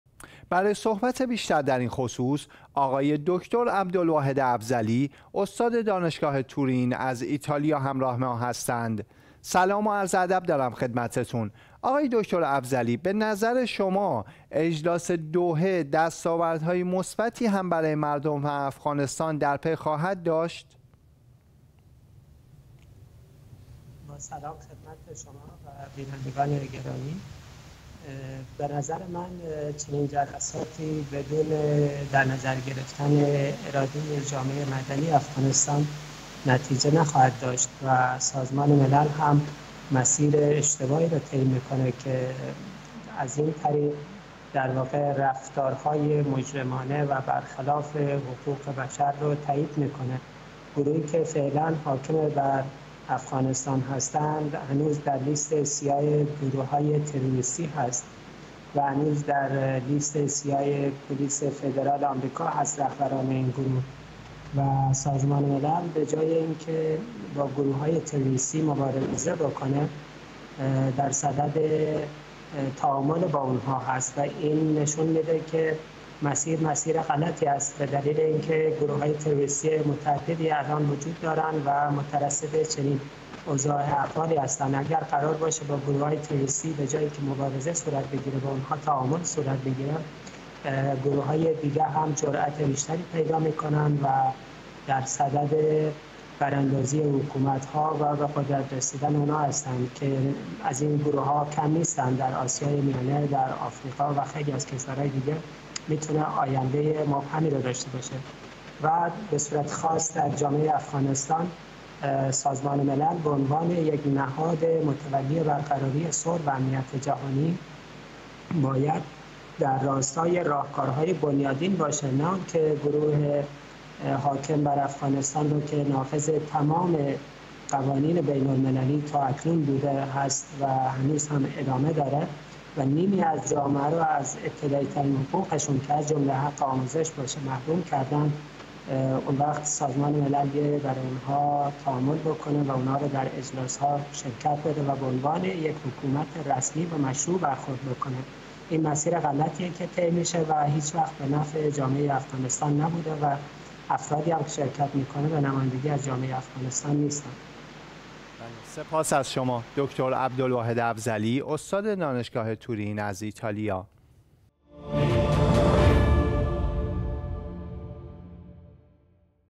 نشست سوم دوحه با نگرانی ها از حذف نمایندگان زنان، اقوام و مذاهب، پایان یافت
گفتگوی اخبار شیعه با 👈دکتر عبدالواحد افضلی، استاد دانشگاه تورين از ایتالیا 🔸🔸